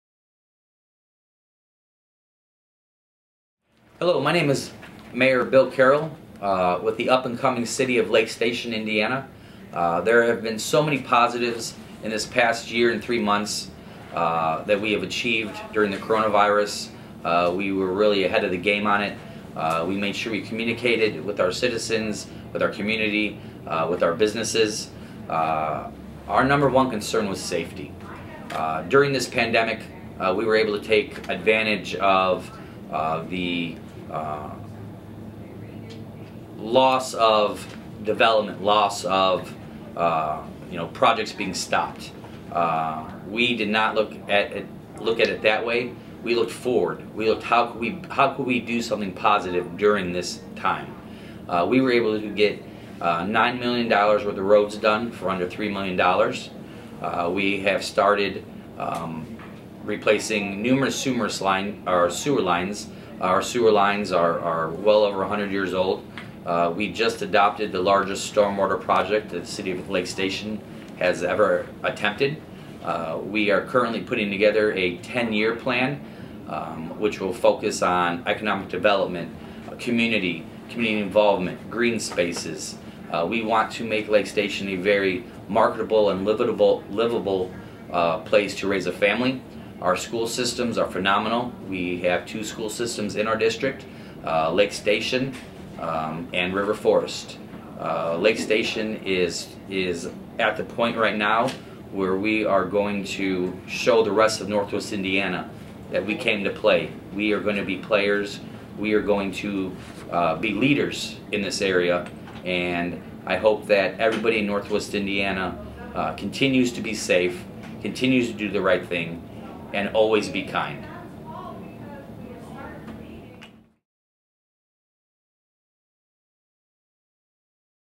WATCH NOW: Bill Carroll speaks about Lake Station's post-pandemic future
Lake Station Mayor Bill Carroll speaks on what the city has to look forward to in 2021.